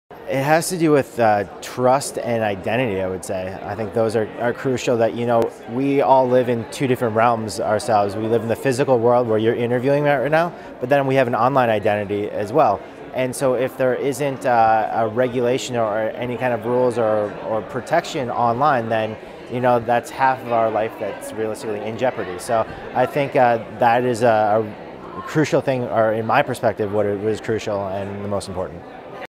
Global IGF 2017: Kyle Shulman on The Key Internet Issue
Kyle Shulman, global project manager for Internet Society, shares their answer to the question, “What is the most important Internet issue to address today for the best tomorrow?” Recorded at the Global Internet Governance Forum 2017 in Geneva, Switzerland, by the Imagining the Internet Center, an initiative of Elon University, North Carolina, USA.